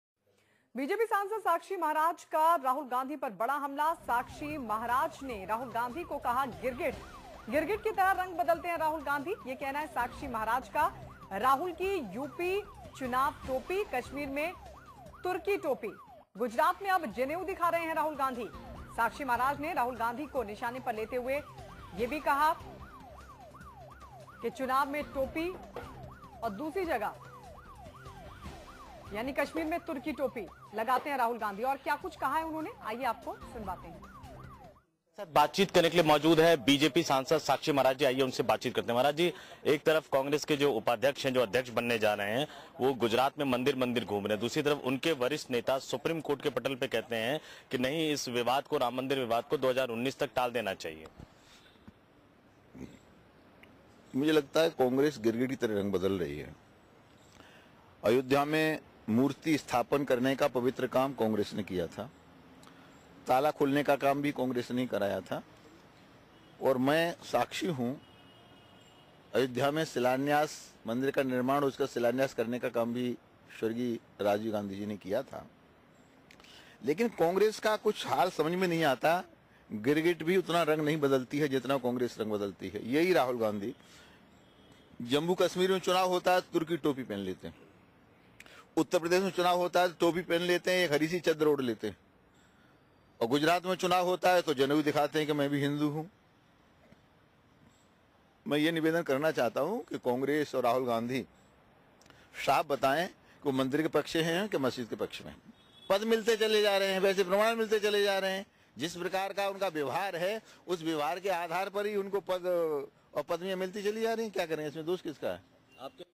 Sakshi Maharaj on Rahul Gandhi by Santosh Pathak
बीजेपी सांसद साक्षी महाराज ने राहुल गांधी को गिरगिट बताया